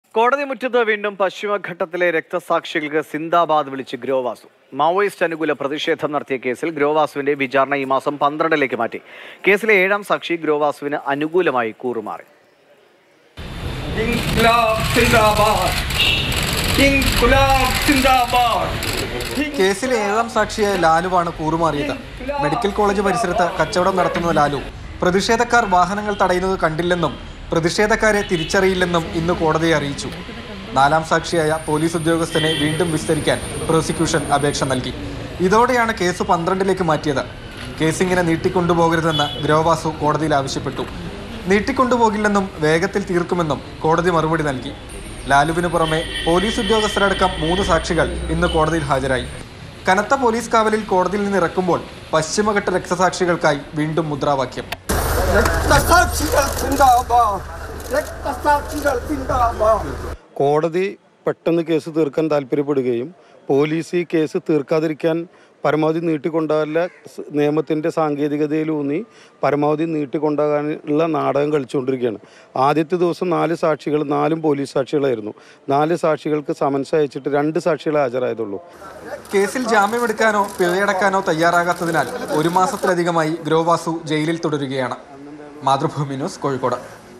രക്തസാക്ഷികൾ സിന്ദാബാദ്; കോടതിമുറ്റത്ത് വീണ്ടും സിന്ദാബാദ് വിളിച്ച് ഗ്രോ വാസു
മാവോയിസ്റ്റ് അനുകൂല പ്രതിഷേധം നടത്തിയ കേസിൽ ഗ്രോ വാസുവിന്റെ വിചാരണ സെപ്റ്റംബർ 12ലേക്ക് മാറ്റി.

Happy viewing!